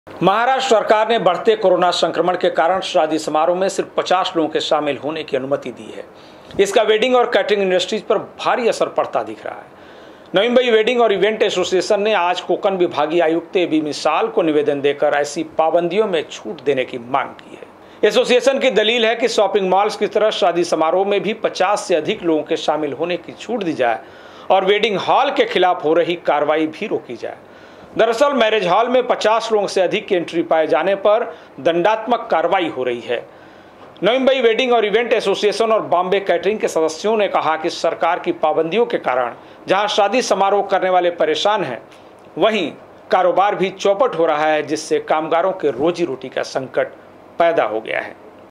पाबंदी हटाने navi mumbai वेडिंग एसोसिएशन ने उठाई मांग
पाबंदी हटाने नवी मुंबई वेडिंग एसोसिएशन ने उठाई मांग